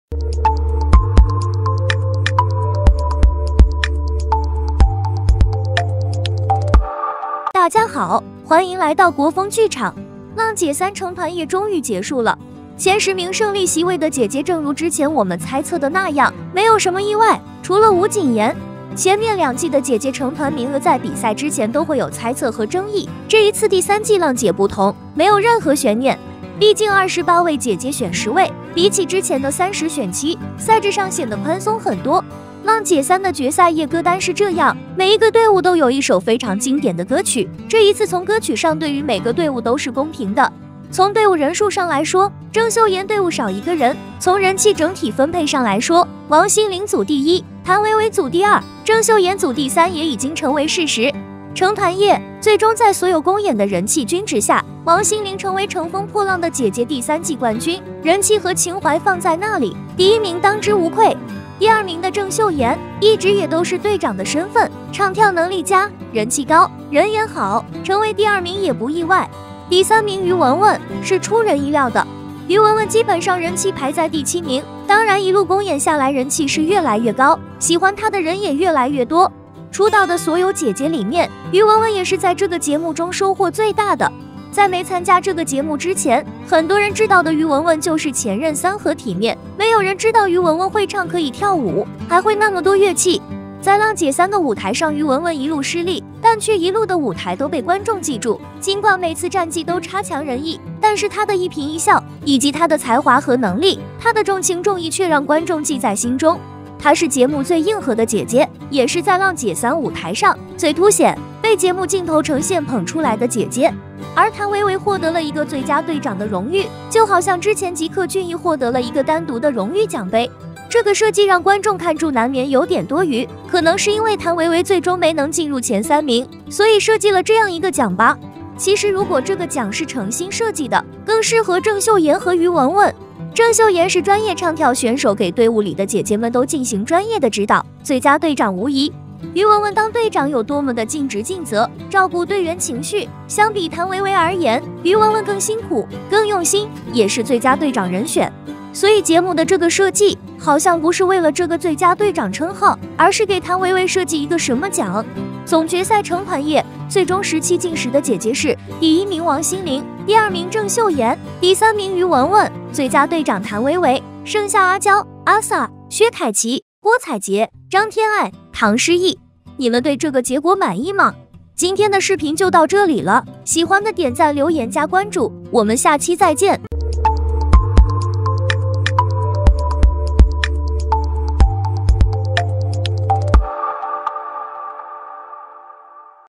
《浪姐3》成團：王心凌冠軍當之無愧，于文文第三是節目“硬捧”！
《浪姐3》成團：王心凌冠軍當之無愧，于文文第三是節目“硬捧”！乘風破浪的姐姐